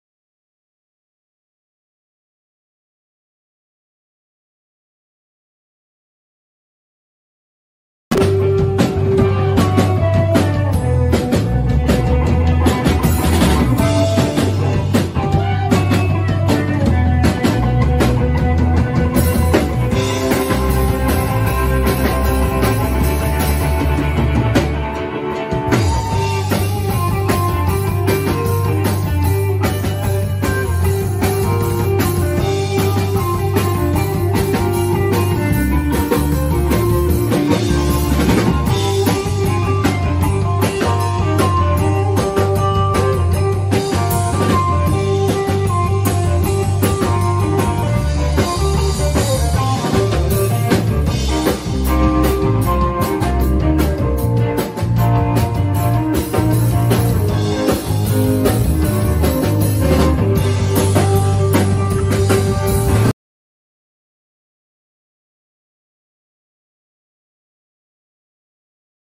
Surf Party at Monkey's Paw
Most recent show at Monkey's Paw Tiki Bar in Springfield Oh-Ree-Gone